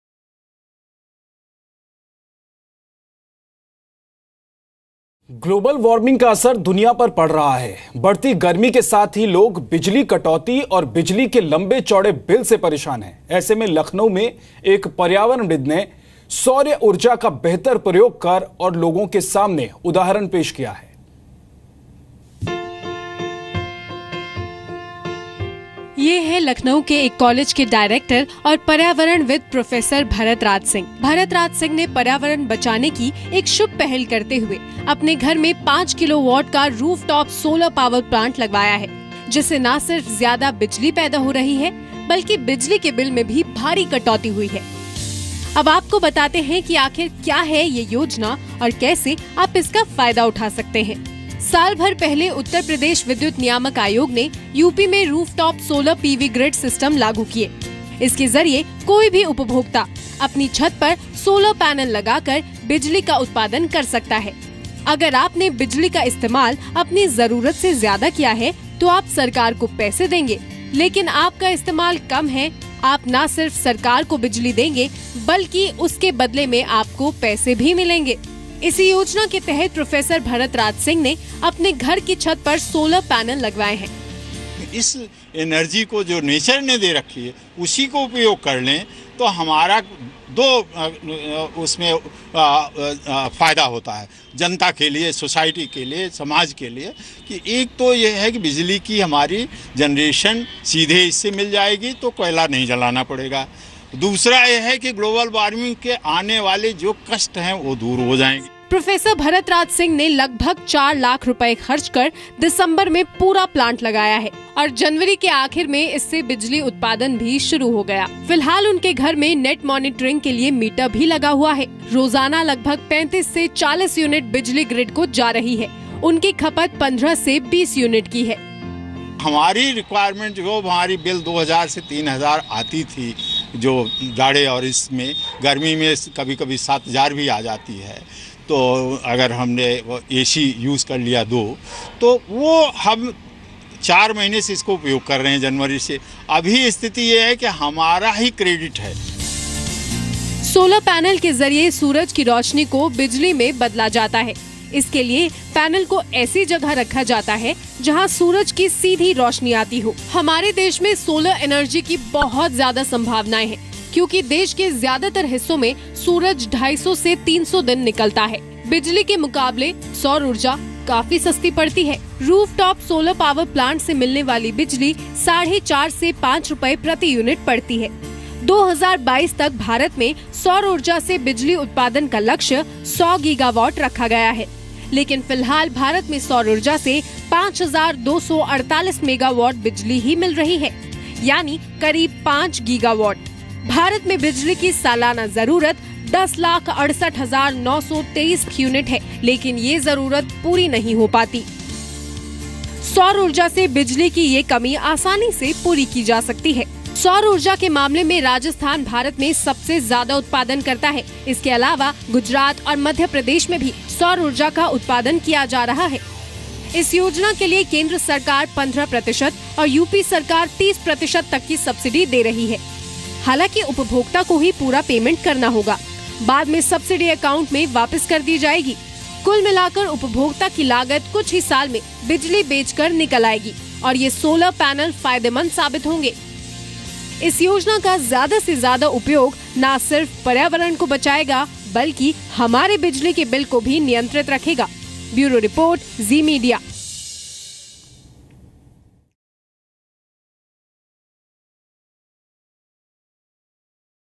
5 किलोवाट ‘सौर ऊर्जा' का लखनऊ शहर मे पहला मकान -Whole house running-ZeeNews-21May2016 at 7:00 PM
लखनऊ शहर का पहला- 5 किलोवाट ‘सौर ऊर्जा' मकान (Watch the amazing story of a college professor who use solar power for his house).
प्रो0 (डा0) भरत राज सिंह, निदेशक व वरिष्ठ पर्यावरणविद, स्कूल ऑफ मैनेजमेण्ट साईसेन्ज जो विगत 15 वर्षों से पर्यावरण असन्तुलन पर समाज में लोगों में चेतना जगाने का कार्य कर रहे है।
प्रो0 भरत राज सिंह ने लखनऊ शहर के आवासीय घरों पर ‘सौर-ऊर्जा’ के उपयोग से पर्यावरण बचाने की एक नयी पहल की है। उ0प्र0 प्रदेश सरकार व केन्द्र सरकार की ‘सौर ऊर्जा नीतियों’ के तहत गोमती नगर क्षेत्र में अपने ‘आवासीय घर’ पर 5 किलोवाट का ‘सौर ऊर्जा उत्पादन का प्लान्ट लगाया गया है। यह कनेक्शन जनवरी के प्रथम-माह में लगवाया गया और अभी तक प्रतिदिन औसतन 20-25 यूनिट सौर-ऊर्जा, विद्युत-पैदाकर ‘पावर कारपोरेशन’ के लाइन में इन्जेक्ट हो रही है। अप्रैल व मई, 2016 के प्रथम सप्ताह में इसका उत्पादन 30-32 यूनिट हो रहा है।
डा0 भरत राज सिंह का कहना है कि लखनऊ में उनकी पहल सर्वप्रथम ‘मकान की छत पर ‘सौर्य ऊर्जा’ उत्पादन का है। अतः वह सभी नगर वासियों से अपील करते है कि विश्व-पर्यावरण संकट को कम करने में, सभी लोग अपनी भागीदारी बढ़ाए और प्रदेश सरकार व भारत सरकार की नीतियों का पूर्ण लाभ उठाकर ‘सौर-ऊर्जा बिजली उत्पादन में लखनऊ को ‘सर्वश्रेष्ठ’ शहर बनाने में सहयोग प्रदान करे।